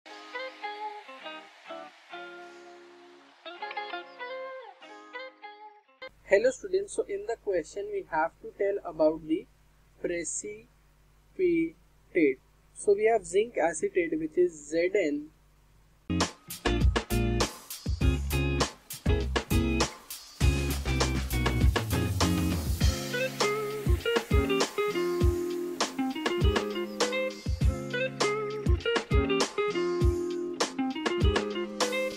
Unknown D Salt Solution is Reactions of the Unknown: Write the molecular, complete ionic, and net i…
Unknown D Salt Solution is Reactions of the Unknown: Write the molecular, complete ionic, and net ionic equations for only the reactions with the precipitates formed with your unknown. If a precipitate did not form with the unknown, then you would not write an equation.